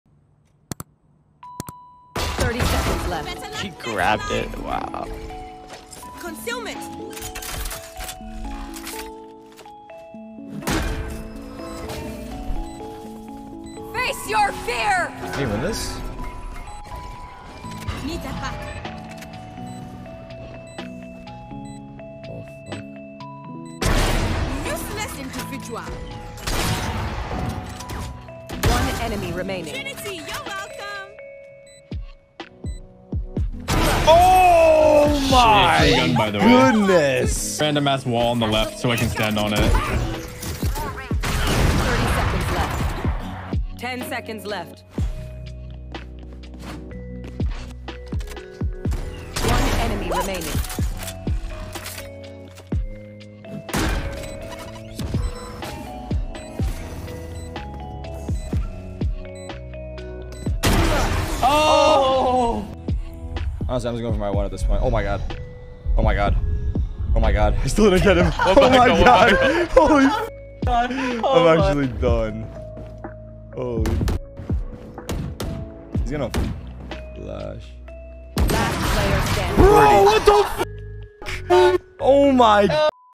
Tarik shook by Timmy's 1v4 clutch, then got 00.06 second clutch himself...
Tarik shocked by Timmy's 1v4 clutch, then got 00.06 second clutch himself...

Support Tarik by following his socials
TTV : @Tarik

If you enjoyed the video, a like & a subscription would be much appreciated ❤❤